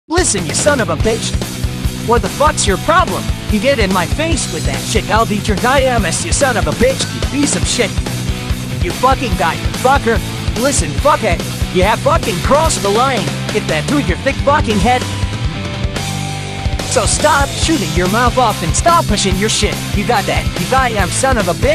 Sonic Says "Listen You Son of a Bitch" but it's Voice to Text
Used the Sonic voice to text from vo.codes to remake this meme.